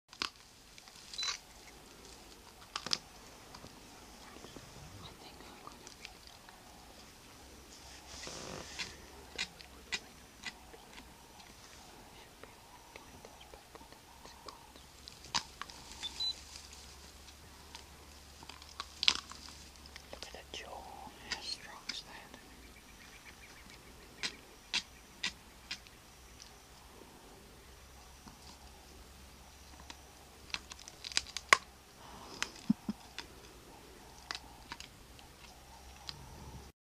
Dombi gnawing on impala bones
Leopard in Timbavati reserve, South Africa